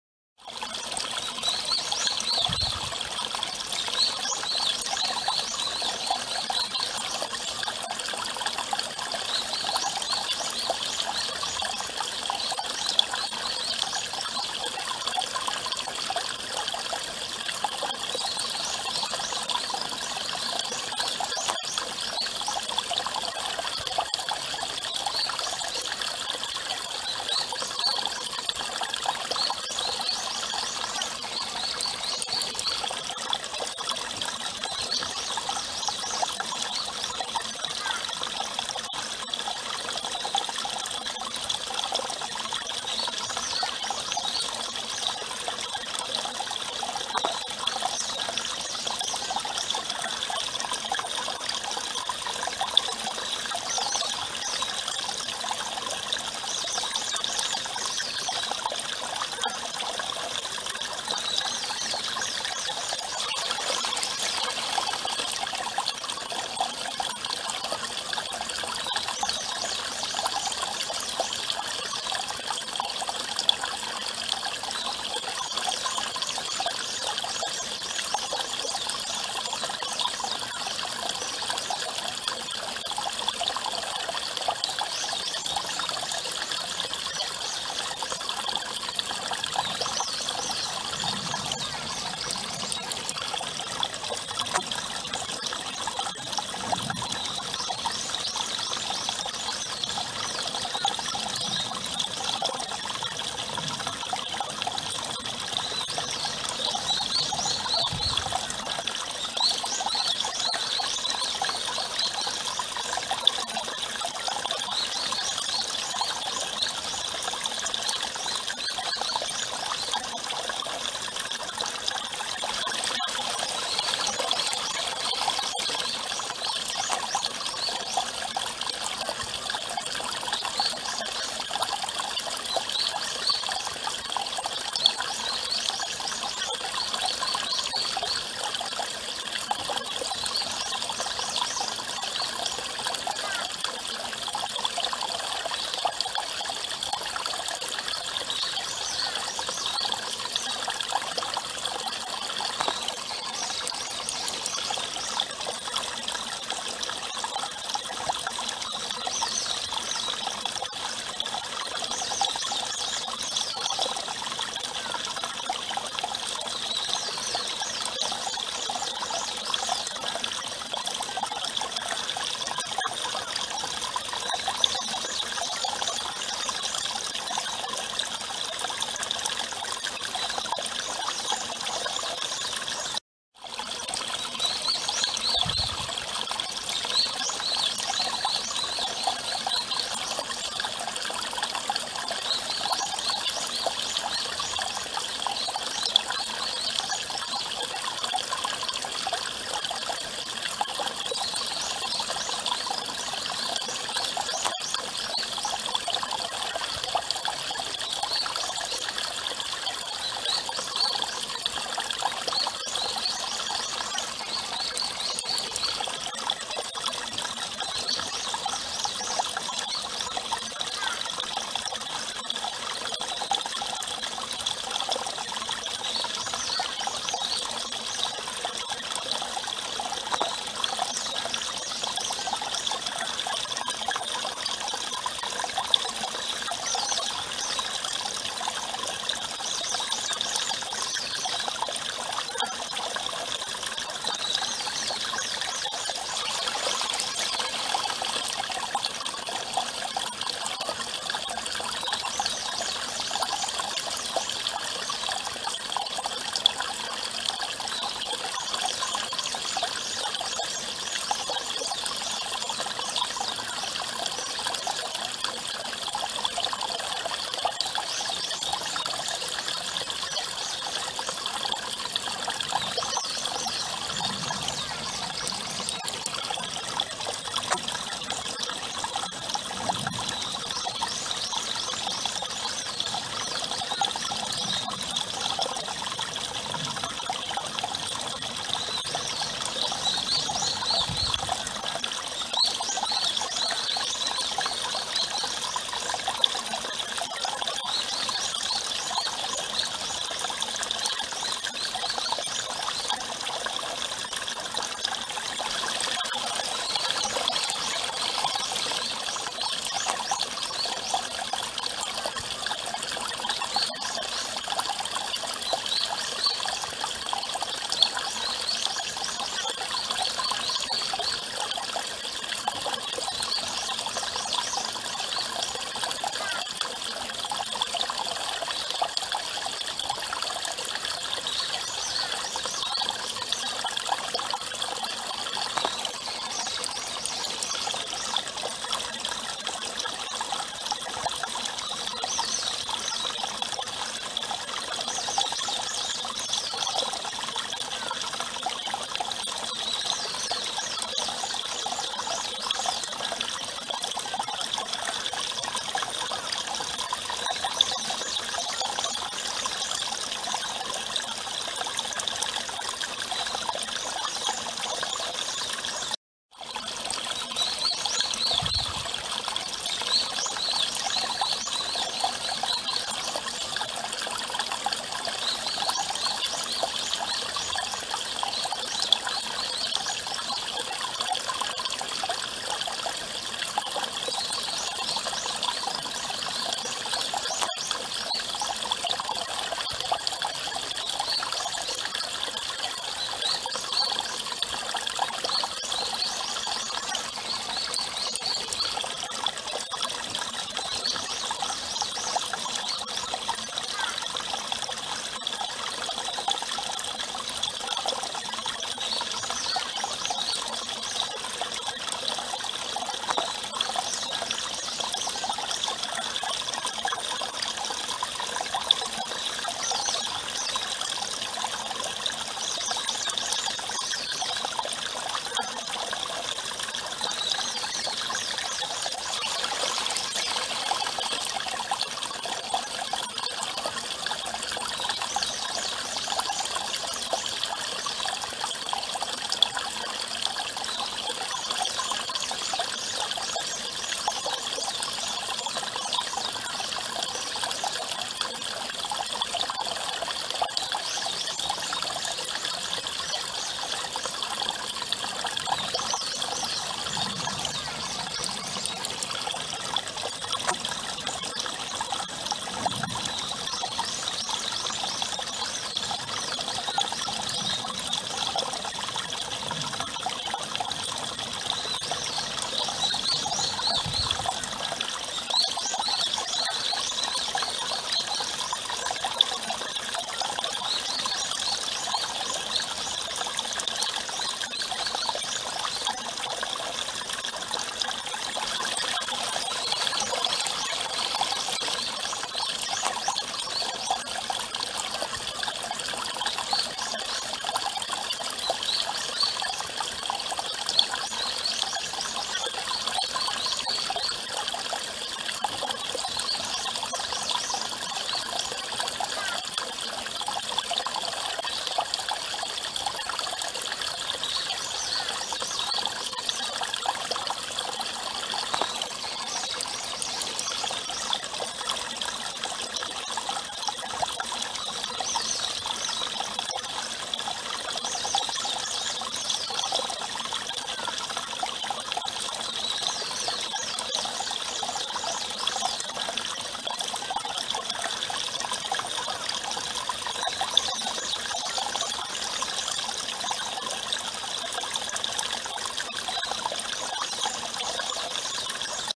마음을 다스리고 싶을 때(16편 양산 천태산 천태사 용왕당&물소리&산새소리 힐링)When you want to control your mind
마음을 다스리고 싶을 때(16편 양산 천태산 천태사 용왕당&물소리&산새소리 힐링)healing
When you want to control your mind (Part 16 Yongwangdang & Water Sound & Mountain Bird Sound in Cheontaesa Temple, Yangsan)